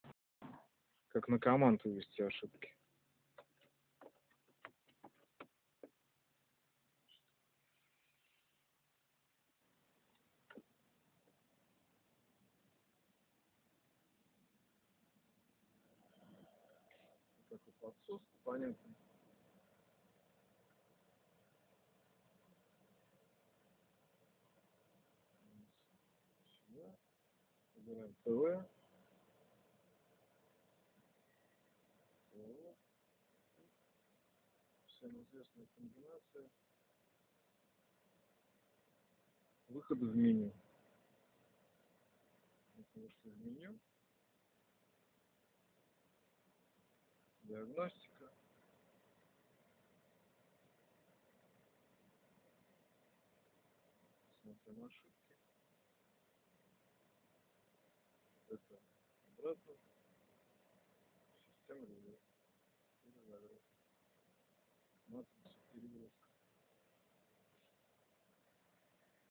вывод ошибок на комманд Mercedes Benz S class W220
Вход в скрытое меню Mercedes-Benz S-class W220 через климат в список ошибок.